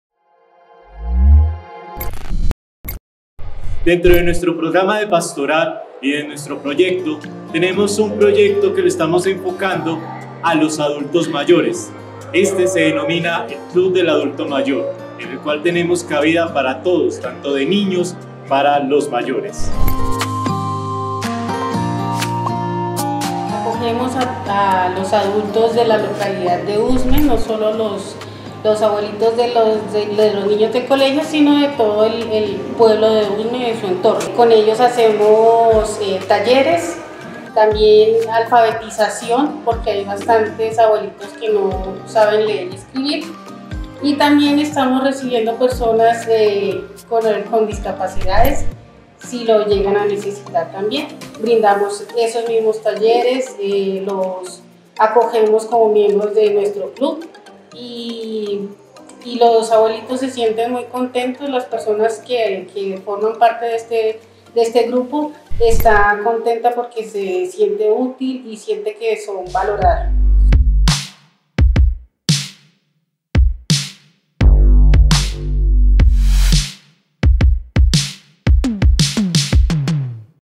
IndivisaClips | Colegio José Eustasio Rivera I.E.D.
IndivisaClips | El Colegio José Eustasio Rivera I.E.D. pone en marcha el "Club para el Adulto Mayor", proyecto pastoral que busca acoger esta población con talleres y procesos de alfabetización.

Producción Gráfica:
Laura Silva
Cinthya Grajales
Producción audiovisual: